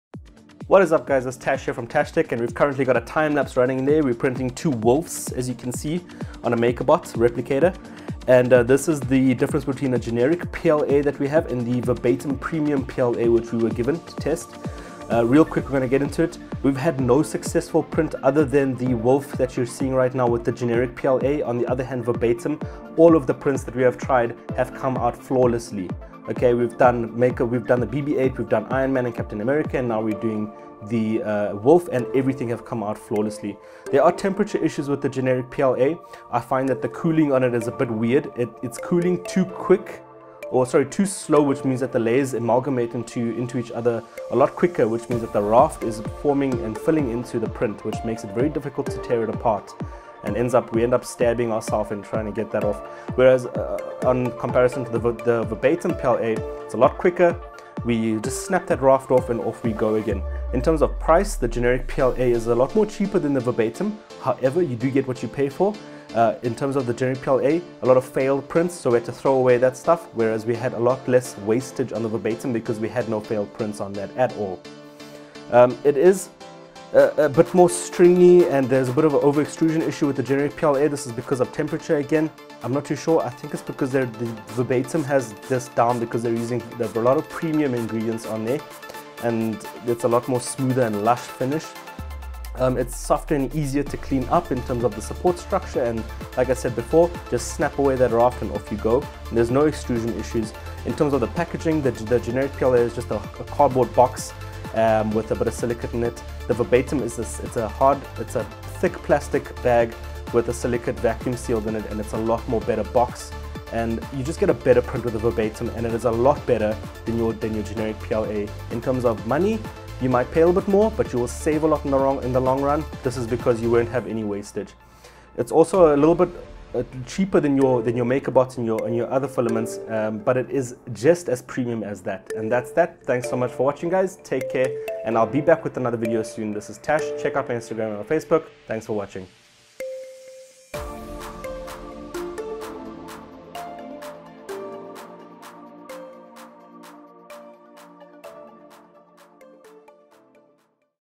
Verbatim vs. Generic 3D printing PLA filament wolves time-lapse
In this time-lapse, view the difference between Verbatim and generic 3D printing PLA filament. Also see how the Makerbot 3D prints these 2 wolves with different filament, as Tash explains the differences.